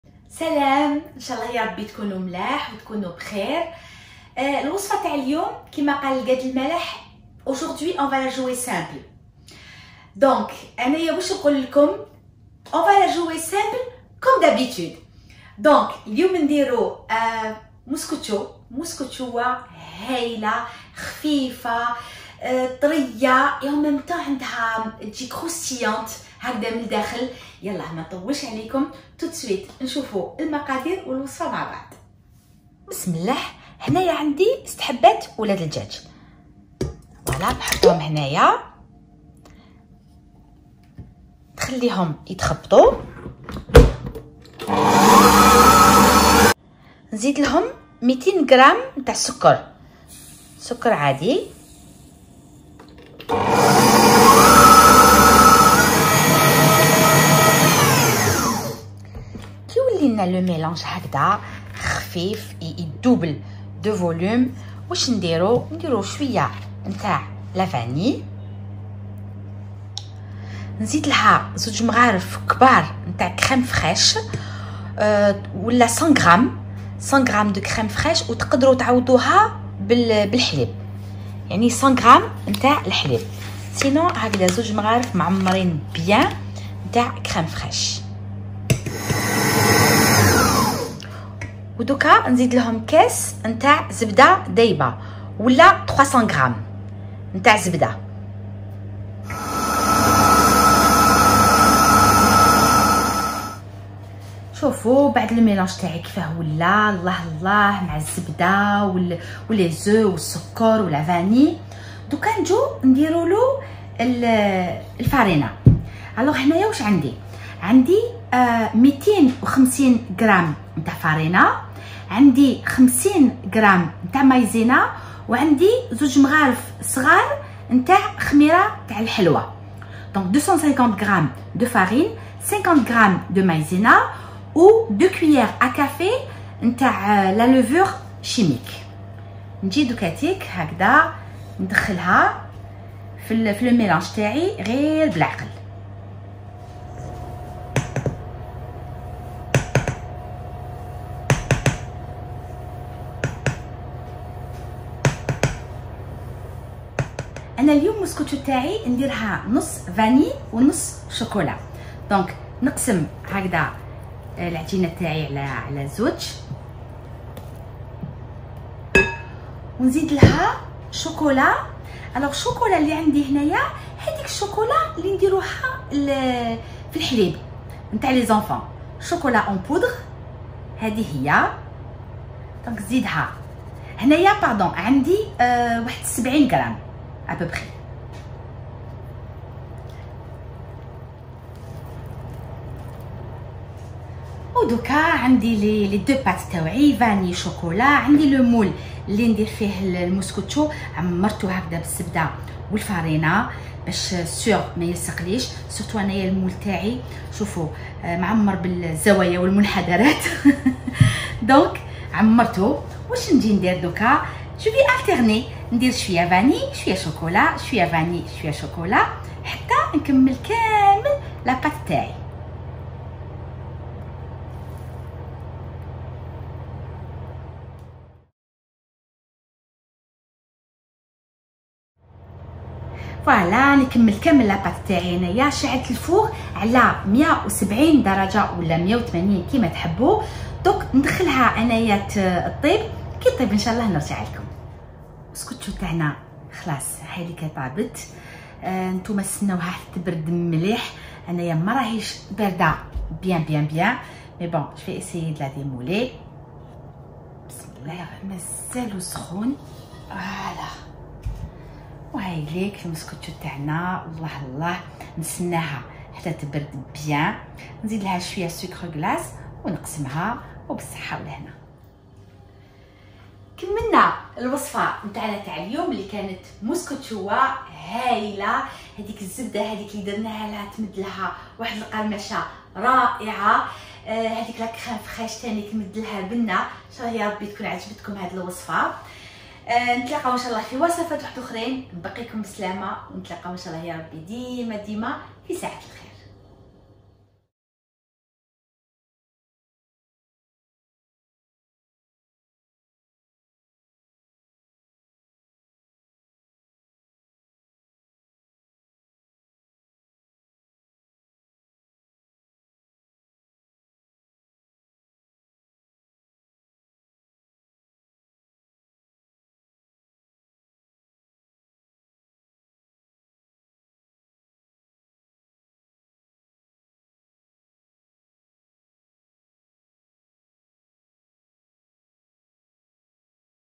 مسكوتشو رائعع ، خفيف و بنين # Mouskoutchou ,delicieux et facile a réaliserer
Les ingrédients
6 œufs
200g de sucre  semoule
100 g de crème fraîche
300g de beurre fondu
Vanille
250g de farine
50 g de maïzena
2 c a c de levure chimique
70 g de chocolate en poudre
Sucre glace pour la décoration